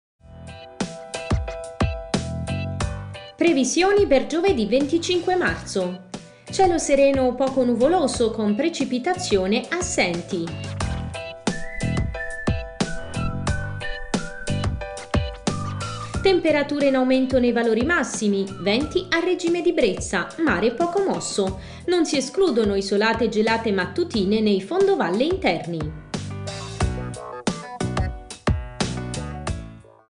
Previsioni meteo per giovedì 25 marzo 2021
Previsioni meteo nelle Marche per giovedì 25 marzo 2021